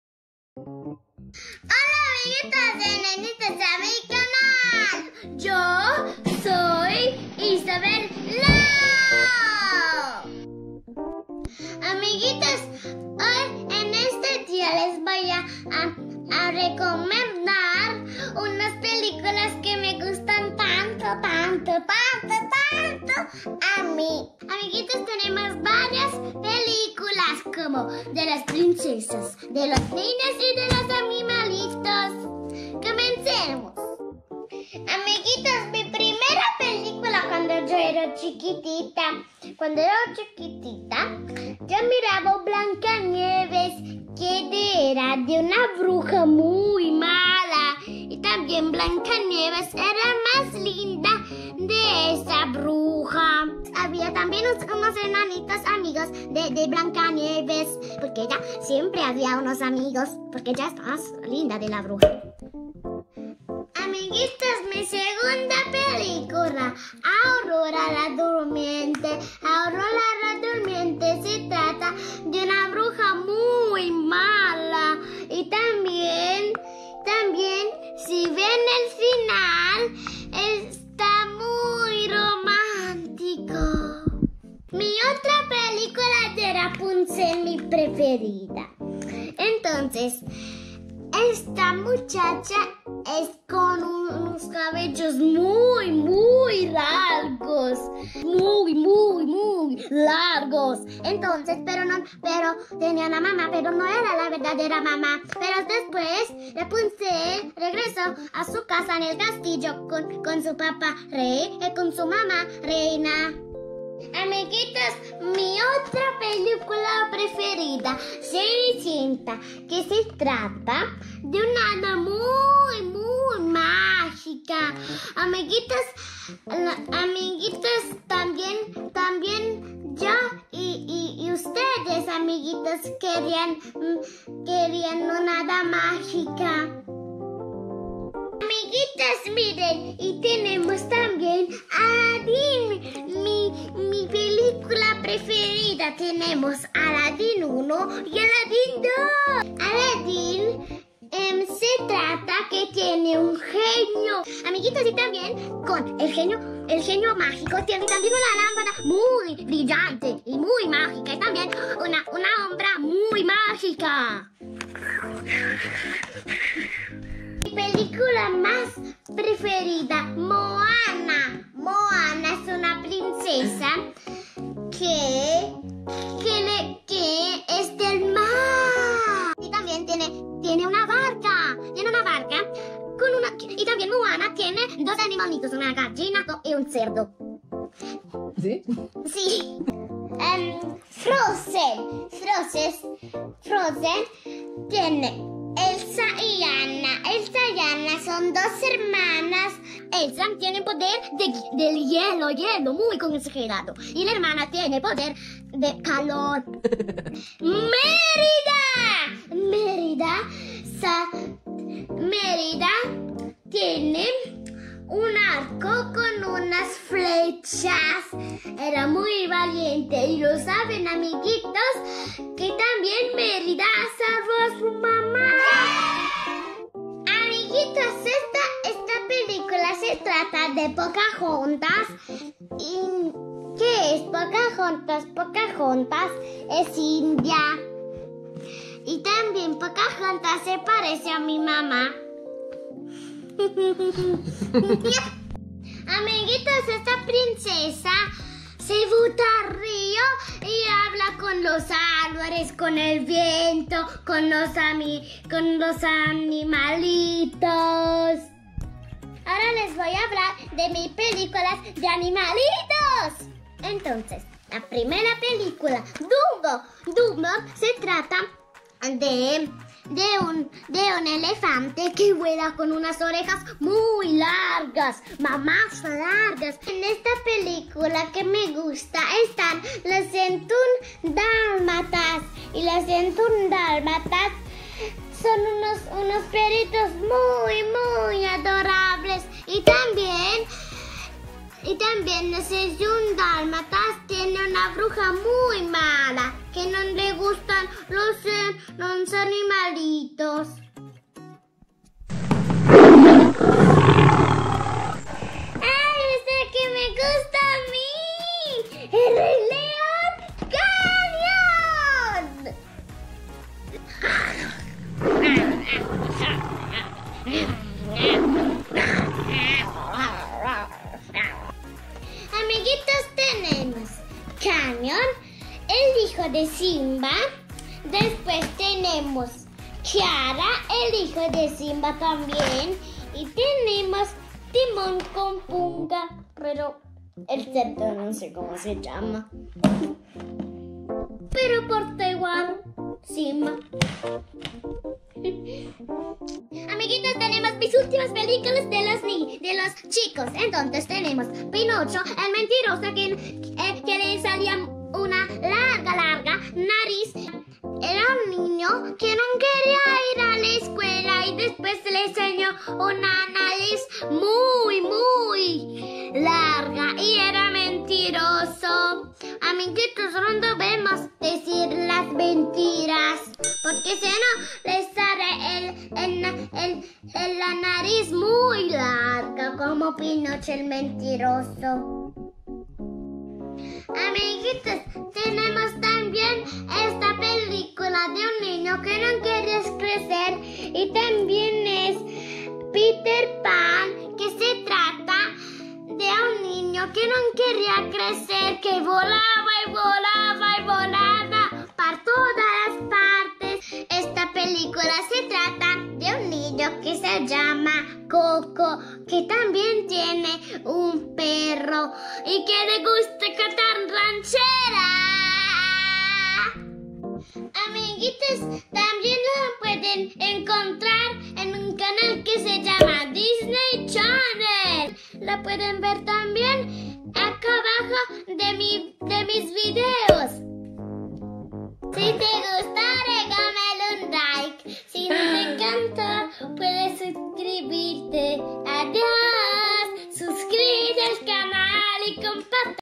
Peliculas para niños|videos para niños|Peliculas Disney|Videos educativos para niños
Amiguitos como debemos estar en la casita les quiero recomendar unas lindas peliculas para niños que puedes ver en casita durante esta cuarentena ,tambien  pueden en ver en este link donde encontrarán Peliculas Disney plus gratis por  7 dias , los quiero muchoteee ,Dios Todopoderoso los bendice!!!😊💕❣💋💋💋💋💋